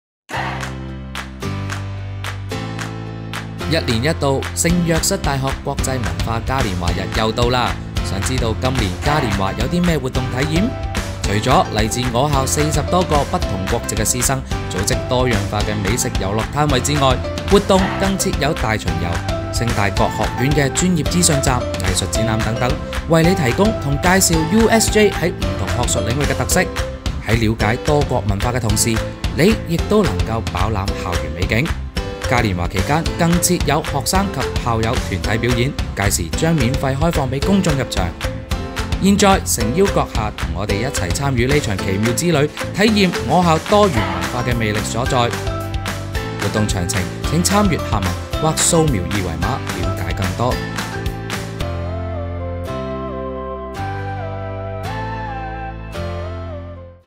USJ International Cultural Carnival 2024 | Coming Soon! (April 20!)
USJ International Cultural Carnival 2024

Immerse yourself in a cultural journey as you explore over 30 diverse cultural booths organised by USJ’s staff, students and alumni members from various nationalities.

Each booth represents a different country, offering you a unique opportunity to experience the richness of global cultures, while indulging in culinary delights from around the world!

聖若瑟大學國際文化嘉年華

聖若瑟大學來自不同國家的教職員工、學生以及校友將組織超過30 多個美食遊樂攤位，讓您感受我校多元文化的魅力所在。